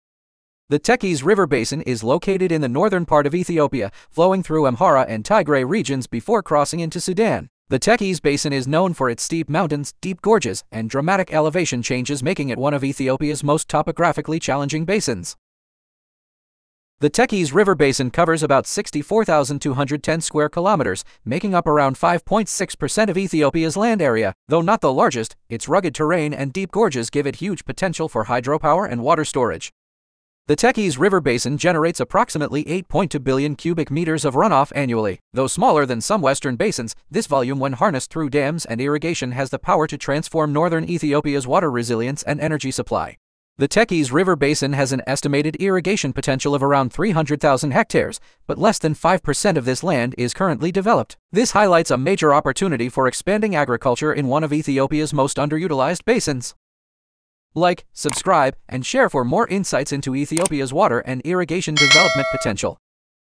Irrigation Potential of Tekeze River Basin Ethiopia
Tekeze River Basin – Ethiopia’s Rugged Water Power Giant
Welcome to Dam & Irrigation Design Hub! In this episode, we explore the Tekeze River Basin—one of Ethiopia’s most dramatic and powerful river systems, flowing through steep gorges and rugged landscapes in the Amhara and Tigray regions.
📍 Located in northern Ethiopia and extending into Sudan, the Tekeze River Basin covers 64,210 km² and is packed with untapped potential in both hydropower and irrigated agriculture.
🔍 What you’ll learn in this video:
✅ Where the Tekeze River Basin is located
✅ Why its mountainous terrain makes it ideal for water storage
✅ How much runoff and irrigation potential it holds
✅ Why this basin is critical to Ethiopia’s water security and energy future
⚡ Key Stats Highlighted:
🌍 Area: 64,210 km² (5.6% of Ethiopia’s land)
💧 Runoff: 8.2 billion m³/year
🌿 Irrigation potential: ~300,000 hectares
🔌 Hydropower potential: 1,500+ MW
🚧 Current capacity: ~300 MW (Tekeze Dam)
🎯 Why It Matters:
The Tekeze River Basin remains largely underdeveloped—yet it holds massive potential to support sustainable development, food production, clean energy, and climate resilience across northern Ethiopia.